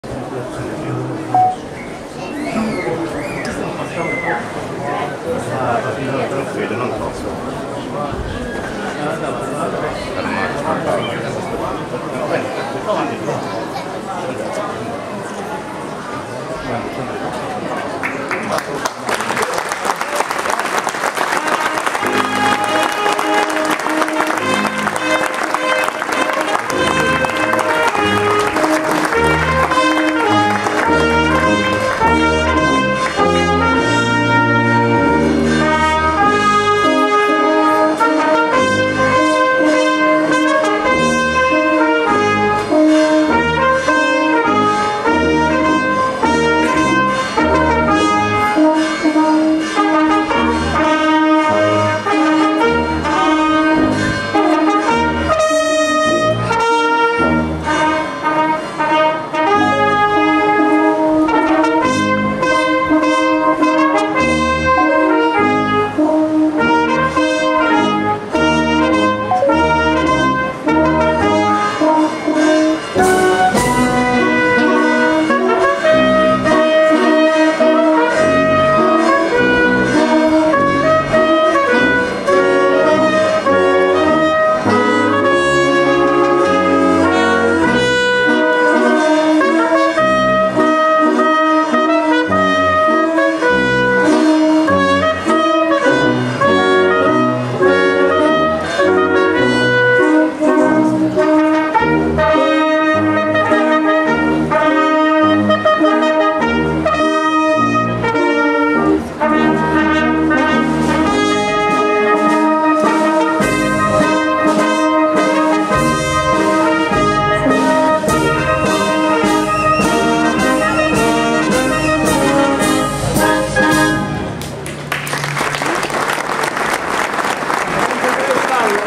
Arena SAN MICHELE ARCANGELO festa 2024. Uscita Processione. 2/5
Arena (VV) Calabria Italia.